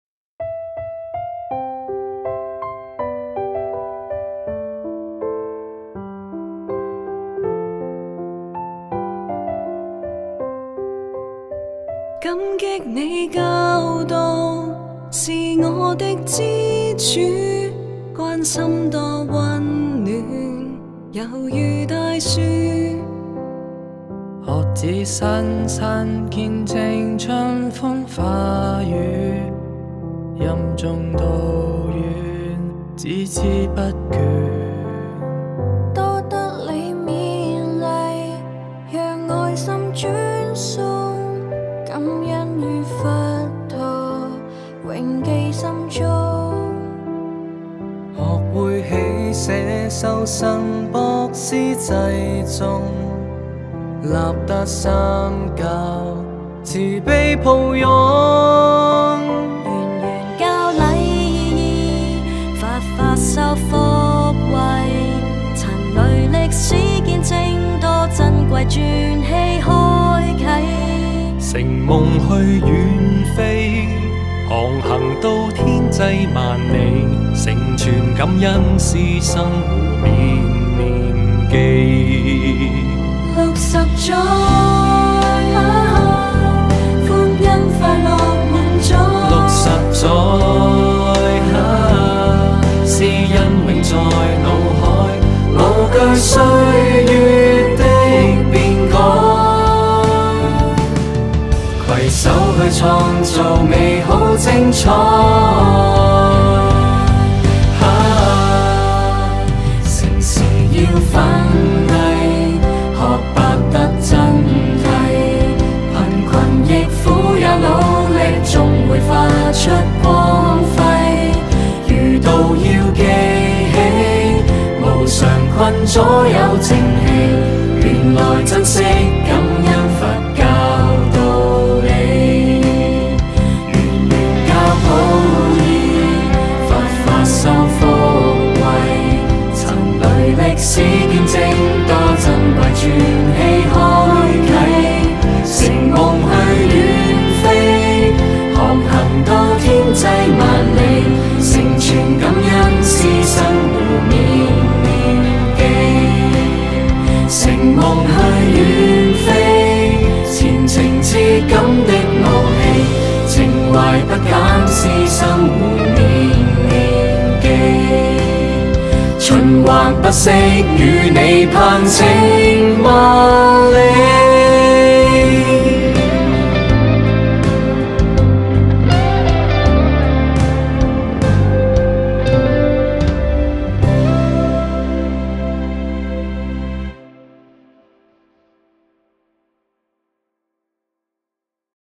60周年鑽禧校慶歷史回顧｜圓玄學院妙法寺內明陳呂重德紀念中學
「感恩承傳 載夢啟航」
適逢60周年校慶，學校由校長帶領，聯同陳琬茵老師、陳子豪老師、一眾校友及學生代表，製作了一首60周年主題曲。與此同時，學校發展及推廣組亦製作了「陳呂60周年歷史回顧」影片，回顧本校60年間發展。